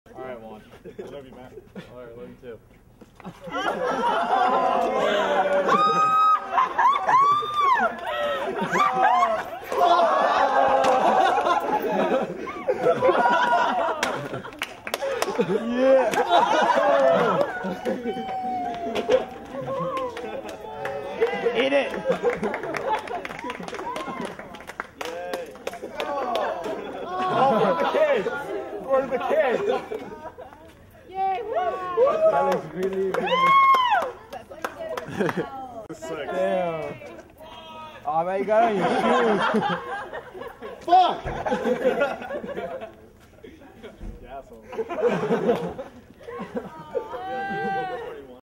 Juan the RA gets pied in the face!
Juan Carlos Lopez is an RA for the 5th and 6th Floor of San Miguel Resident Hall at UCSB. His floors raised enough money for a fund raiser and now he has to get pied in the face 4 times.